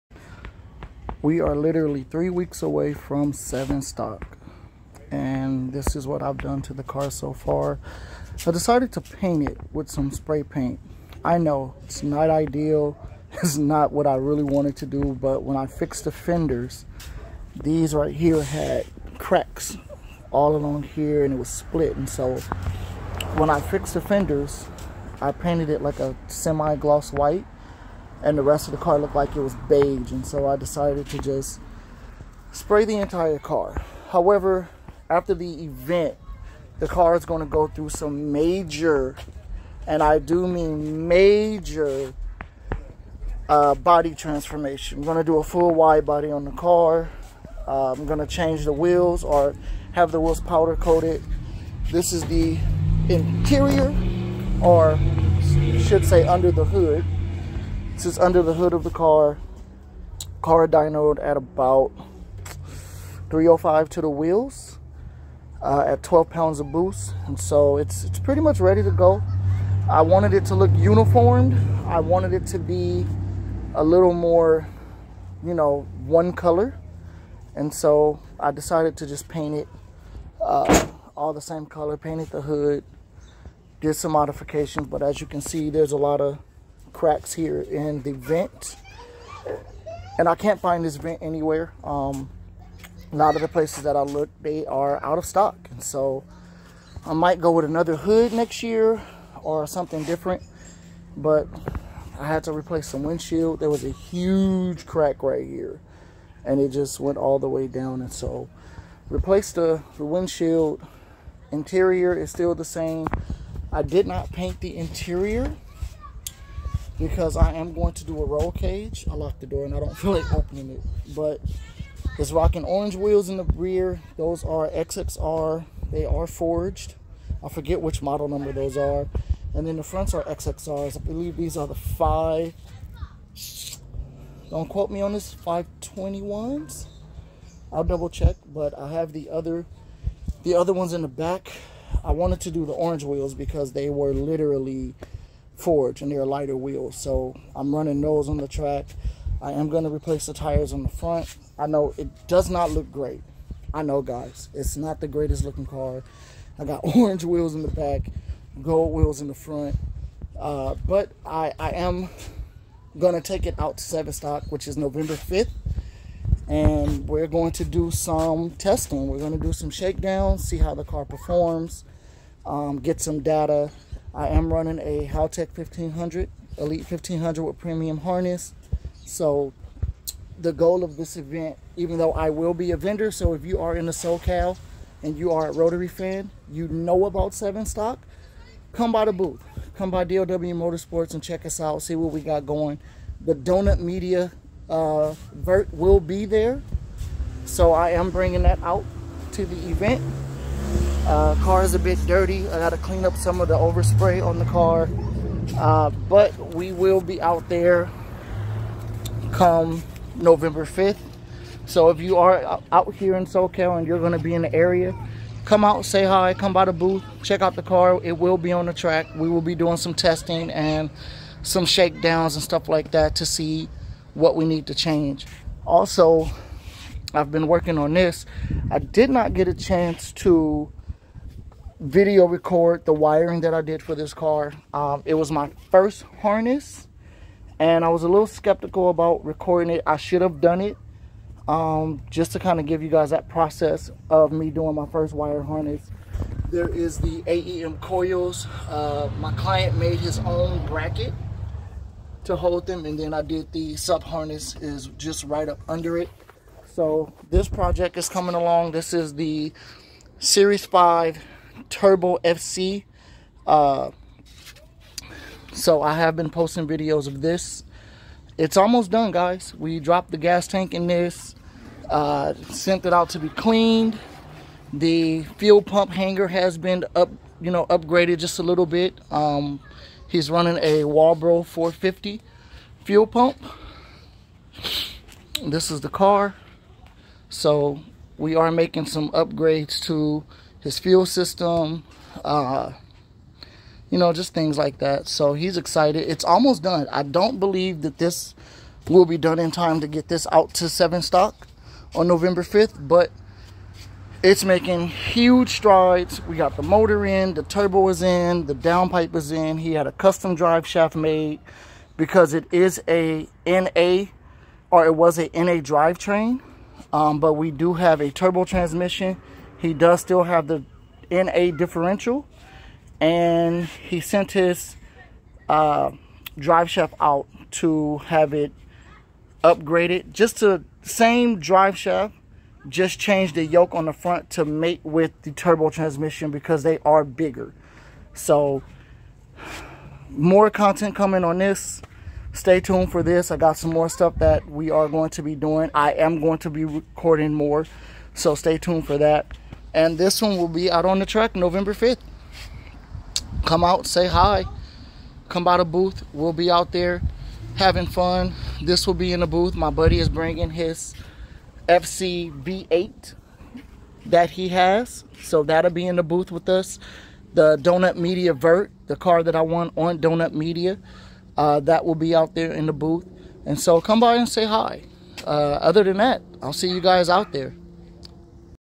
Will we make it to SevenStock 24?
In this video we prep the car for track day for November 5th